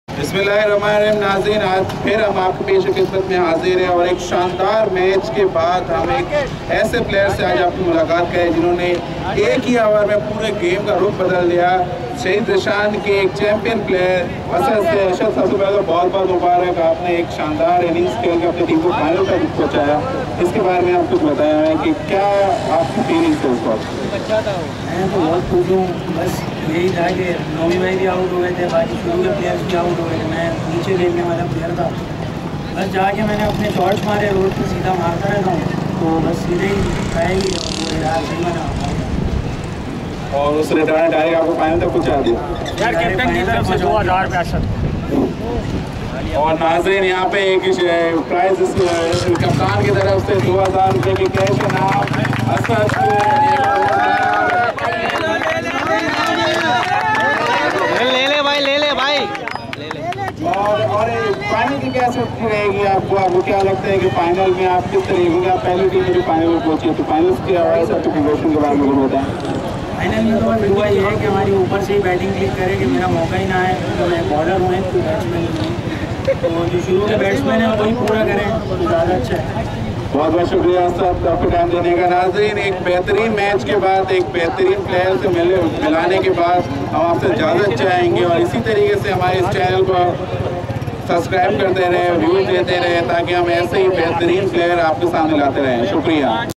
Interview With Asad (Shaheed Zeeshan) Brilliant Batting Against (Ghaznavi Sports)
1st All Layari Mohtarma Shaheed Benazir Bhutto cricket tournament 2017